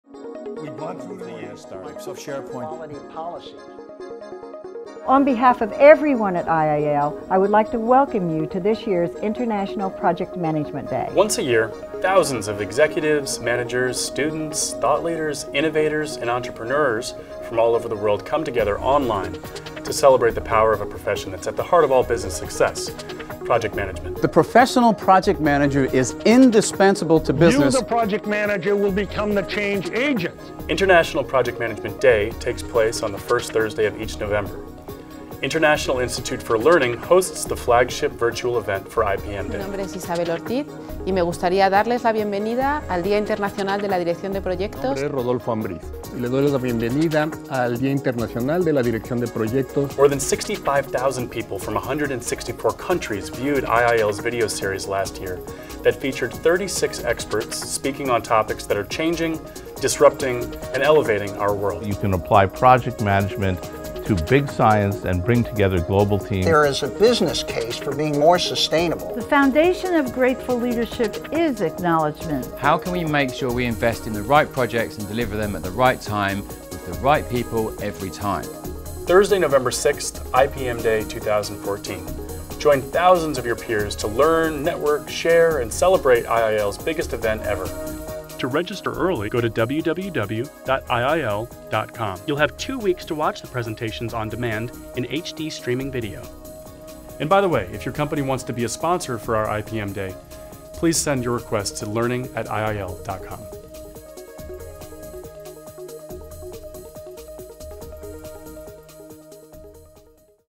International Project Management Day 2014 Trailer
Trailer for IIL's International Project Management Day 2014: Power of the Profession.